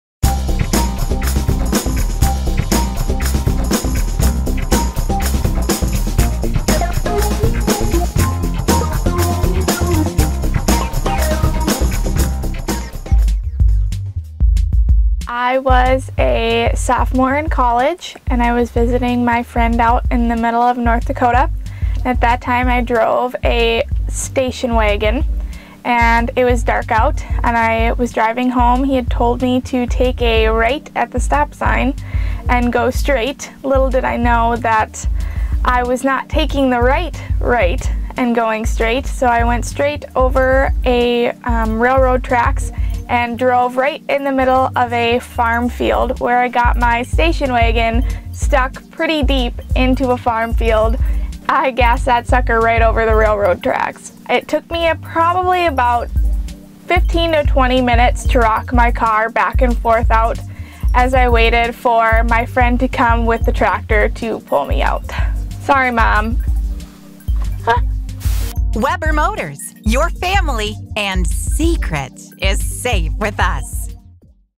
Webber Family Motors Backseat Confession: Someone Get The Tractor
Make sure you take the right "right" turn, or risk ending up in a cornfield.